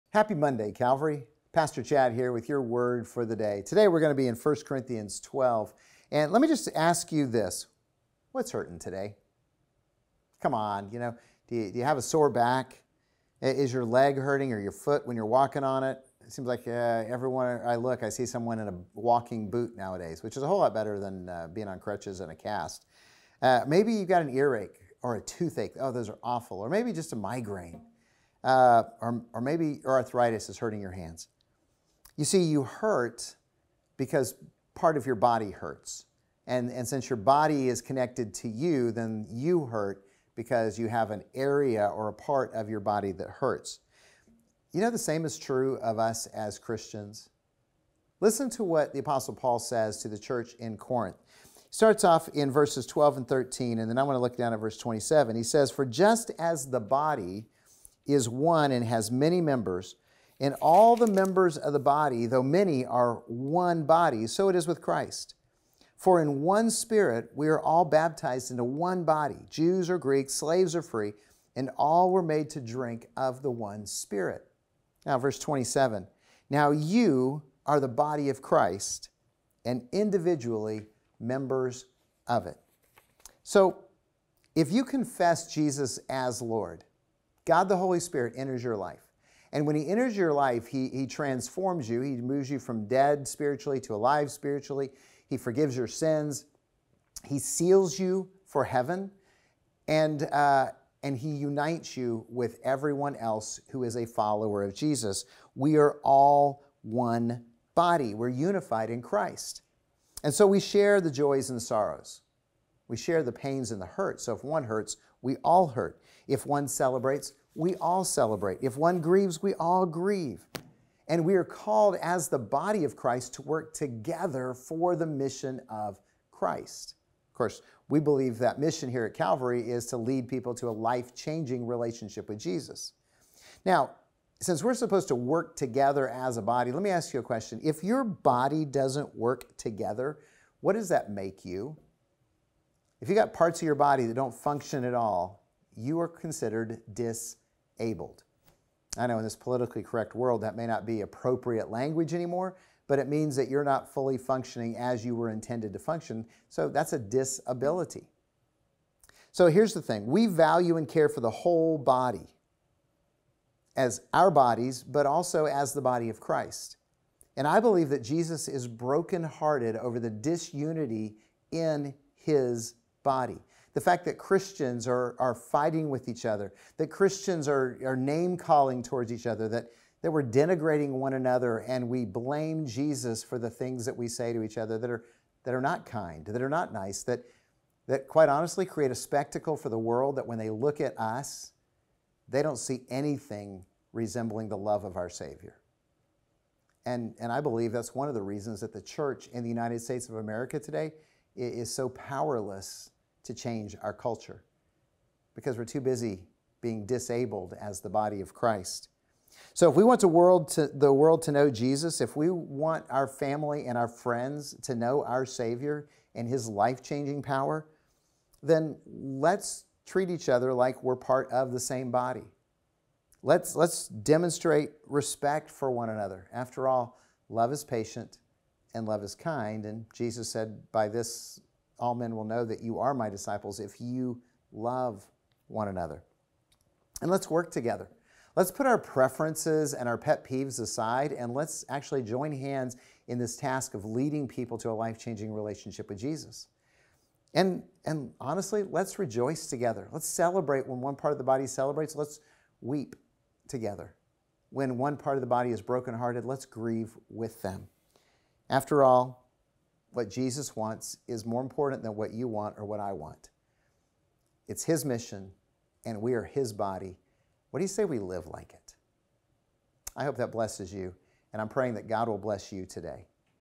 Your Word for the Day - Episode 545
You’ve likely heard it said that we are the body of Christ, today Pastor Chad helps explain what that means.